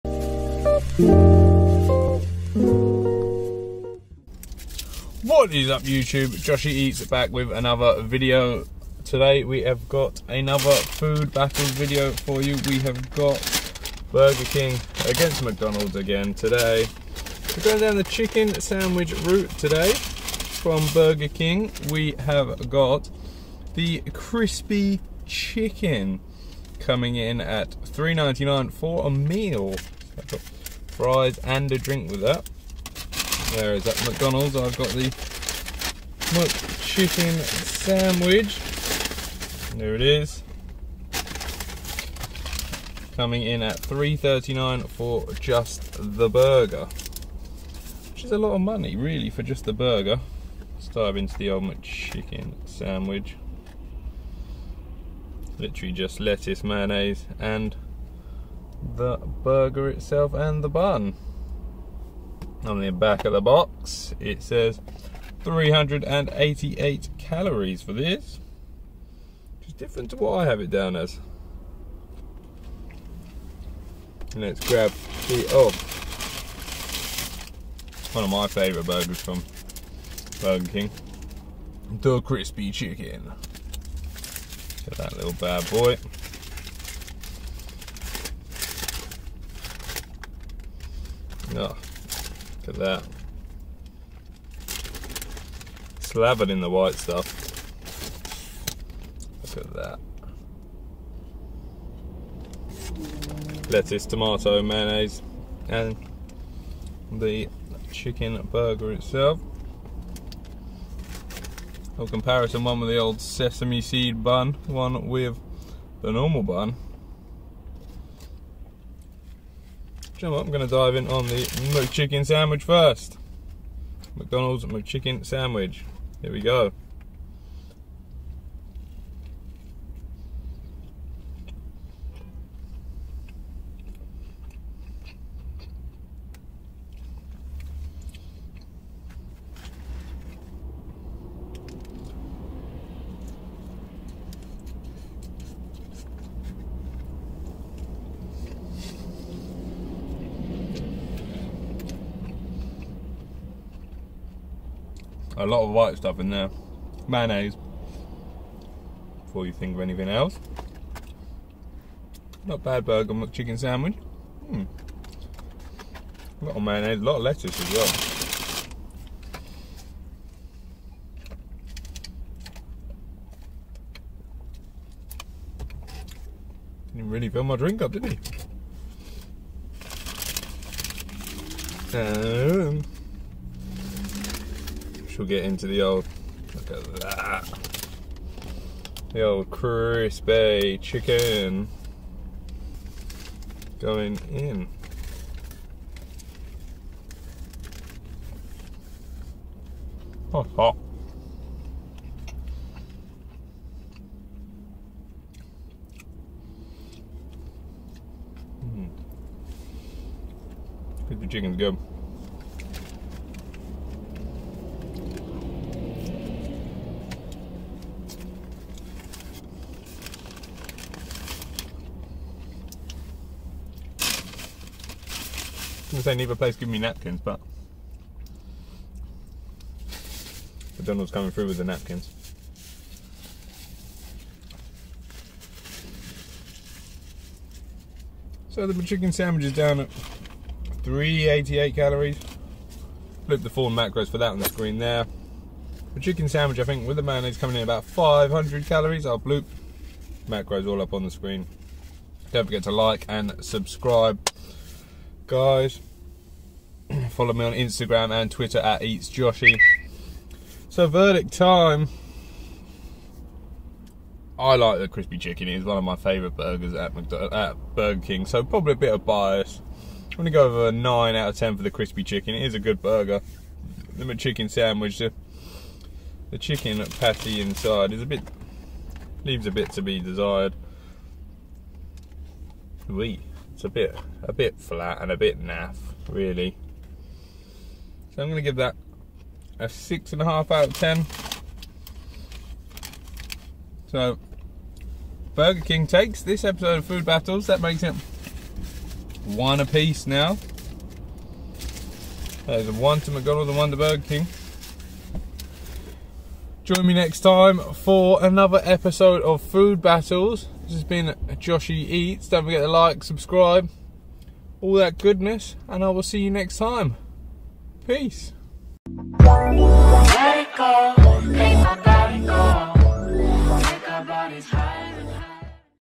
McDonalds Vs Burger King - Ep.3 Chicken Sandwich - McChicken Sandwich vs Burger King Crispy Chicken
McDonalds vs Burger King Food Battles Episode 3, we have the McChicken Sandwich vs The Crispy Chicken.

DIVE IN THE FIND OUT WHO WON EPISODE 3!